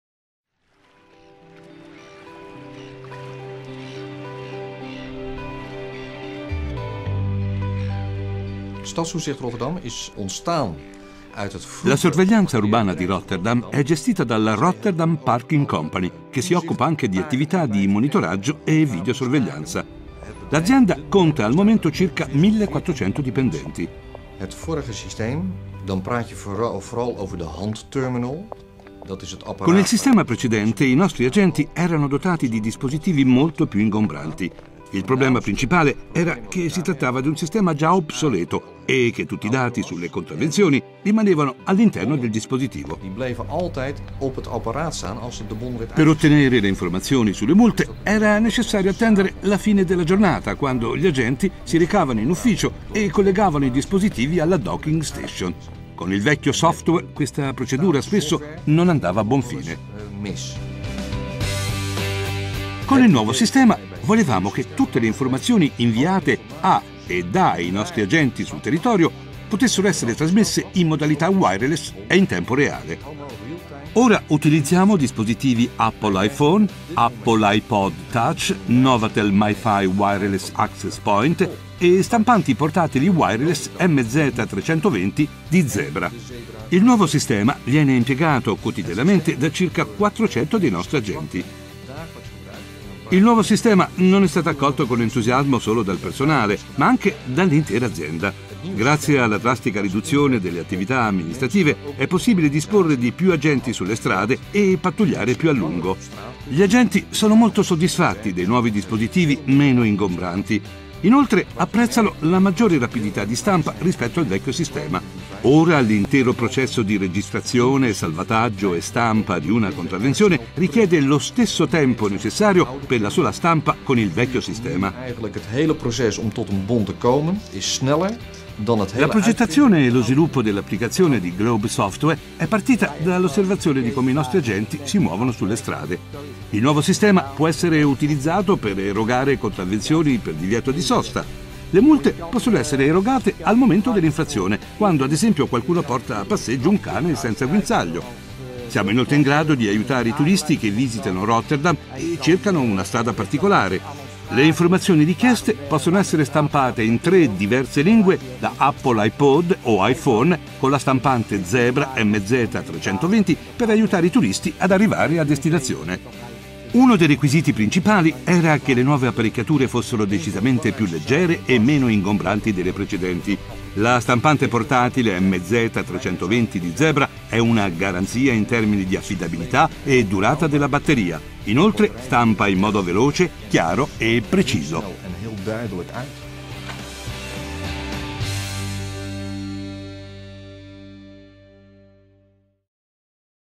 City of Rotterdam (Italiano) | Zebra
Stadstoezicht Rotterdam impiega stampanti portatili Zebra MZ 320 per la stampa di contravvenzioni per divieto di sosta. Per saperne di più su Zebra Technologies, visita